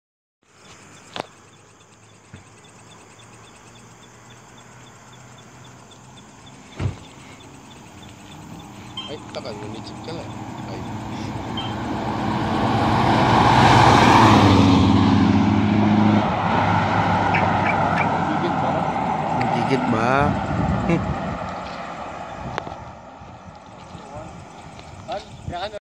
Test Run 400M at Beluran, Vigo 2.5 Unichip Vs Colorado 2.8 Remap
Hilux Vigo 2.5 non vnt stock + Unichip vs Chevrolet Colorado 2.8 Stock REMAP